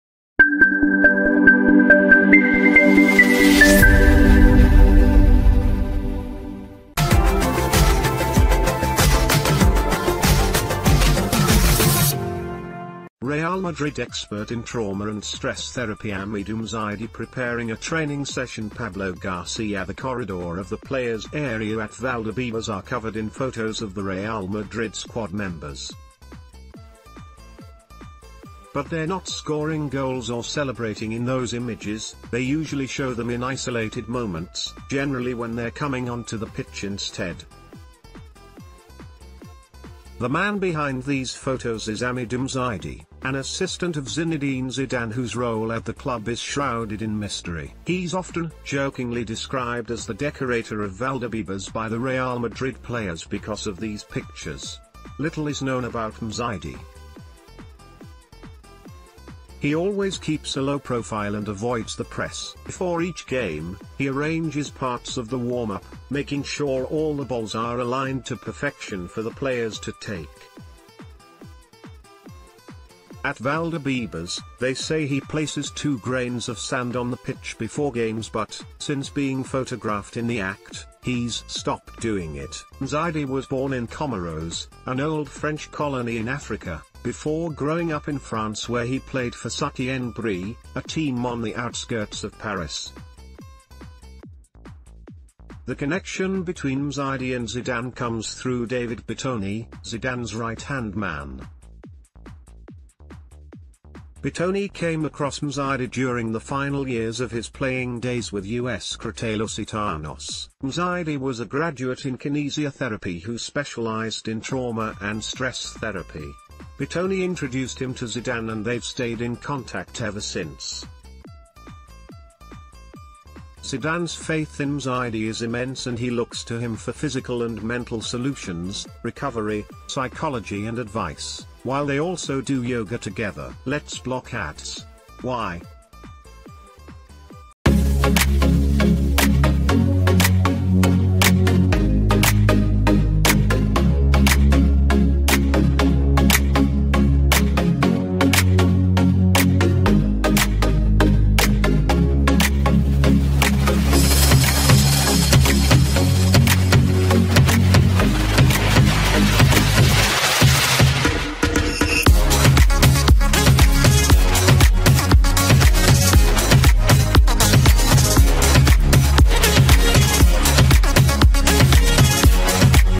Meet Hamidou Msaidie, Zidane's assistant who decorates Valdebebas- Real Madrid news today
Real Madrid Expert in trauma and stress therapyHamidou Msaidie preparing a training session Pablo GarcíaThe corridor of the players' area at Valdebebas are covered in photos of the Real Madrid squad members. But they're not scoring goals or celebrating in those images - they usually show them in isolated moments, generally when they're coming on to the pitch instead. The man behind these photos is Hamidou Msaidie, an assistant of Zinedine Zidane whose role at the club is shrouded in mystery. He's often jokingly described as the "decorator of Valdebebas" by the Real Madrid players because of these pictures. Little is known about Msaidie. He always keeps a low profile and avoids the press. Before each game, he arranges parts of the warm-up, making sure all the balls are aligned to perfection for the players to take. At Valdebebas, they say he places two grains of sand on the pitch before games but, since being photographed in the act, he's stopped doing it. Msaidie was born in Comoros, an old French colony in Africa, before growing up in France where he played for Sucy-en-Brie, a team on the outskirts of Paris. The connection between Msaidie and Zidane comes through David Bettoni, Zidane's right-hand man. Bettoni came across Msaidie during the final years of his playing days ...